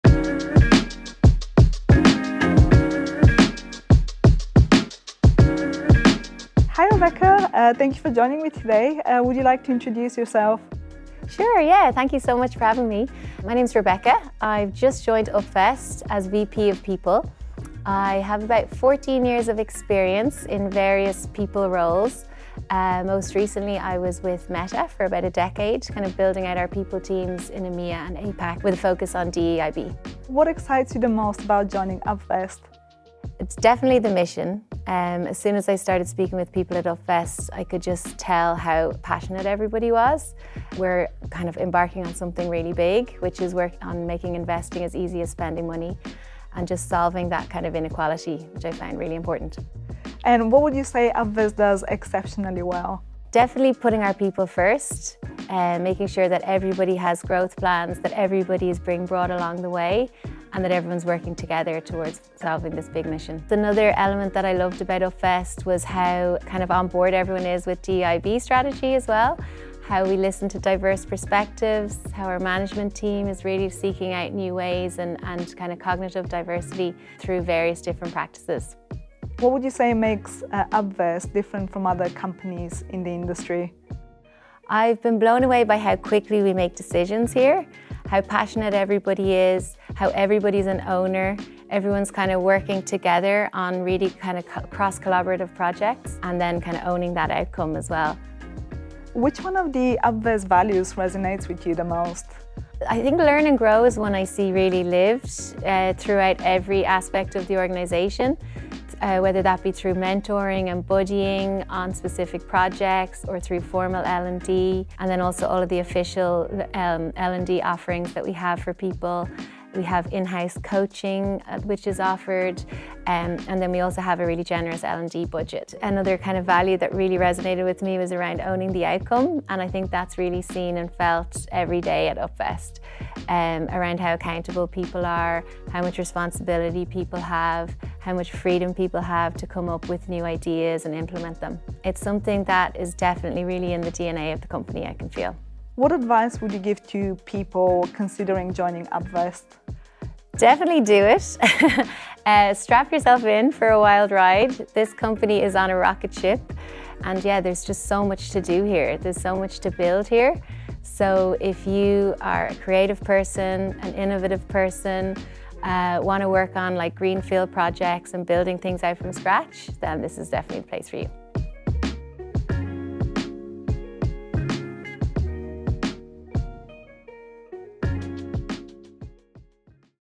Inside Upvest: Rebekah Fox's insights on company culture and values
Step behind the scenes of one of Europe's most innovative fintechs with Rebekah Fox, VP People at Upvest. In this exclusive interview, Rebekah shares her unique perspective as a recent addition to the Upvest leadership team, offering a fresh look at what makes Upvest's company culture truly special.